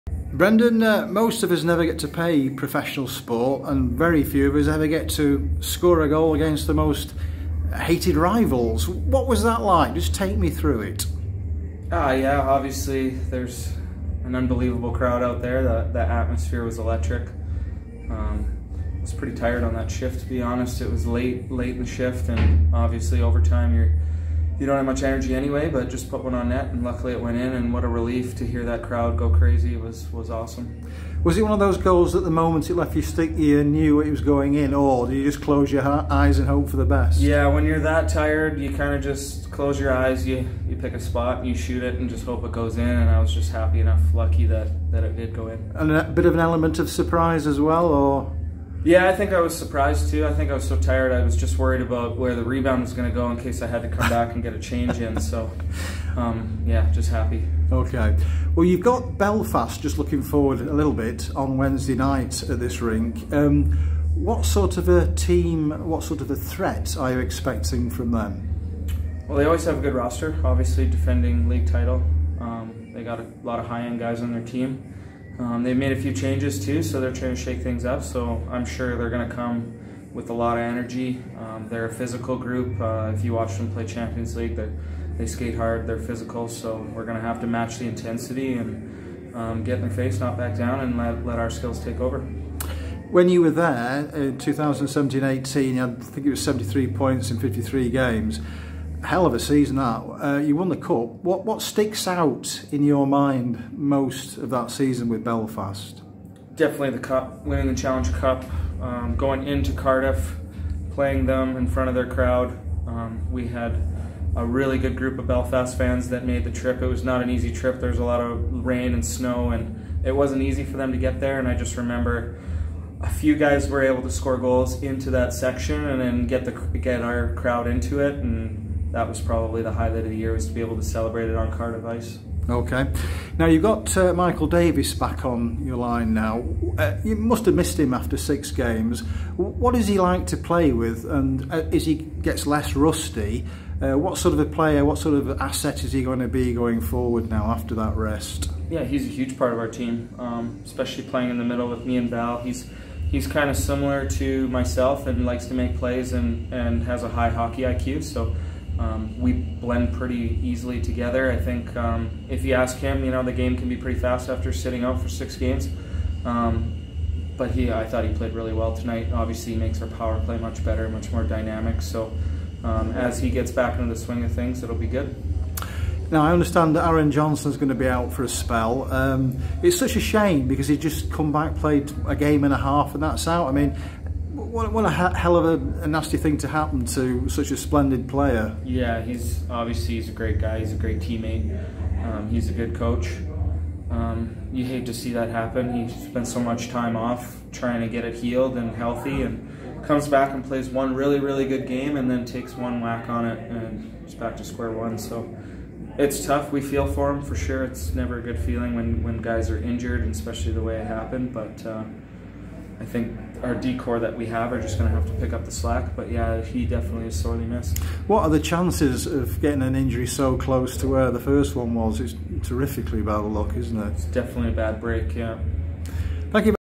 What's it like to score against your bitter rivals?
Sheffield Steelers race's view after Nottingham win